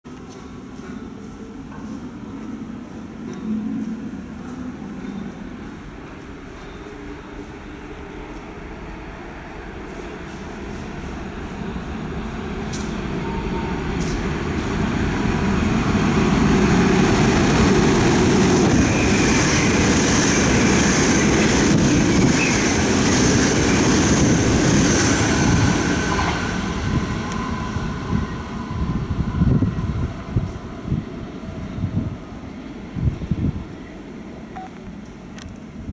2x VIRM komt door Station Rijswijk
2x VIRM komt door Station Rijswijk als Intercity richting Delft.